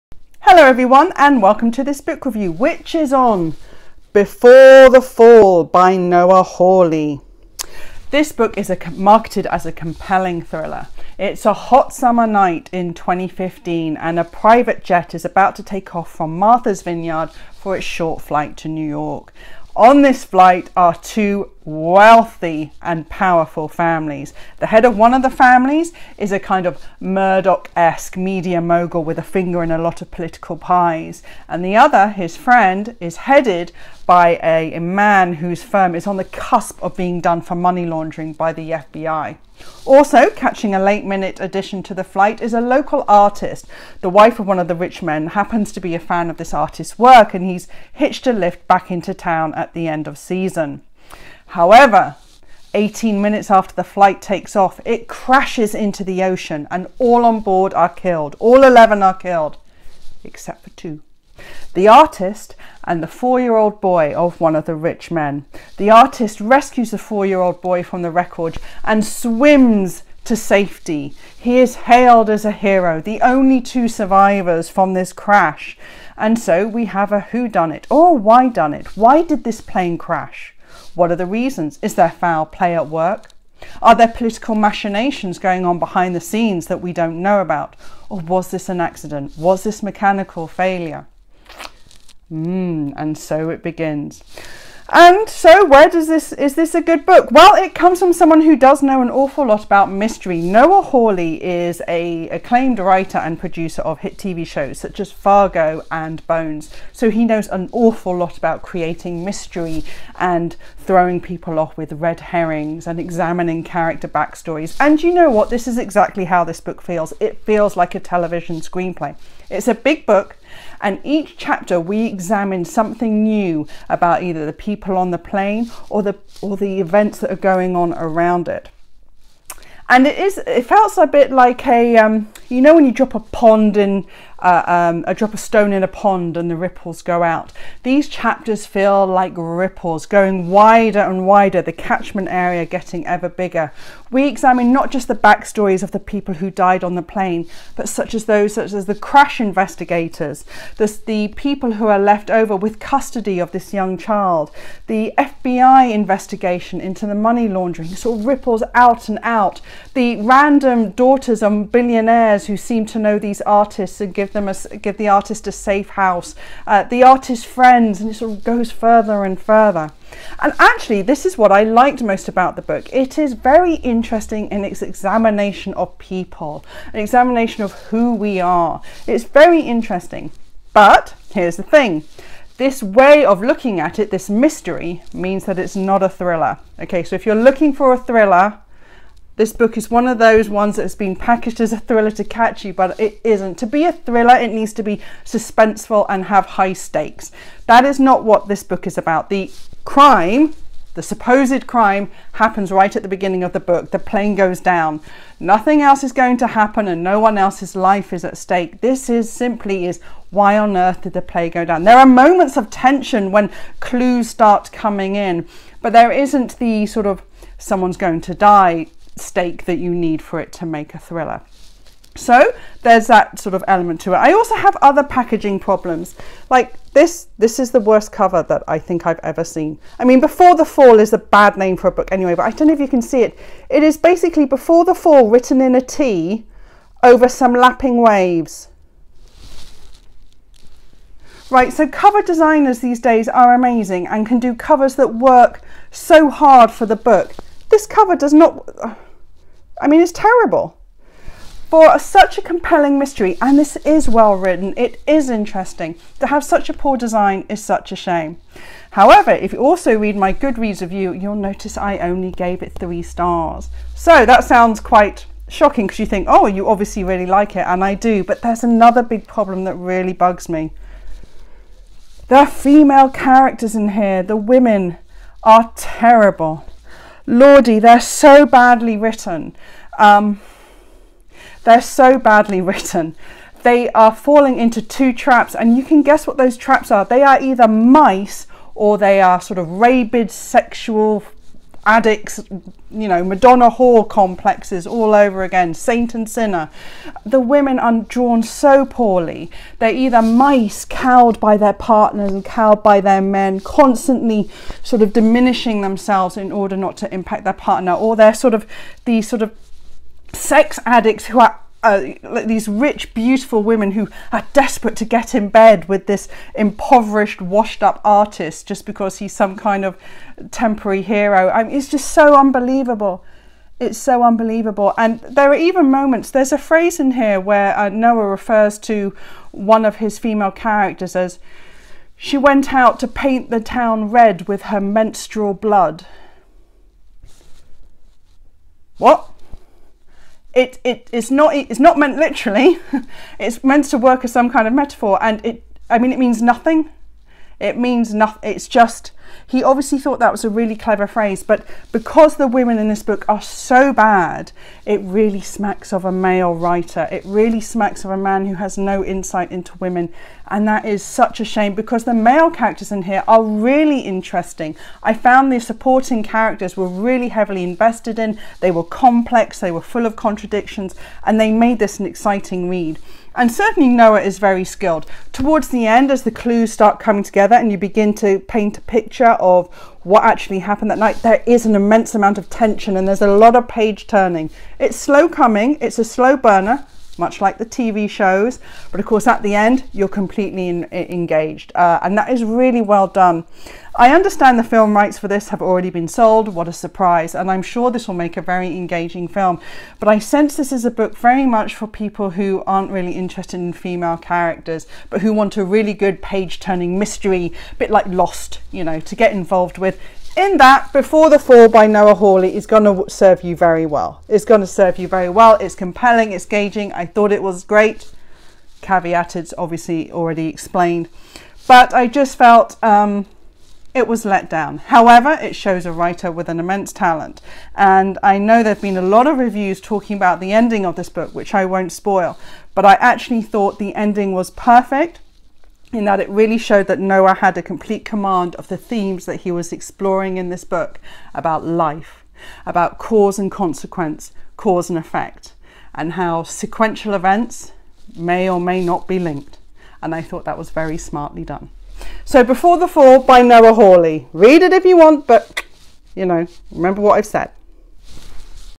Victoria's Book Review: Before the Fall by Noah Hawley
On a foggy summer night, eleven people--ten privileged, one down-on-his-luck painter--depart Martha's Vineyard headed for New York. Eighteen minutes later, the unthinkable happens: the passengers disappear into the ocean. The only survivors are Scott Burroughs--the painter--and a four-year-old boy, who is now the last remaining member of a wealthy and powerful media mogul's family. So, what happened?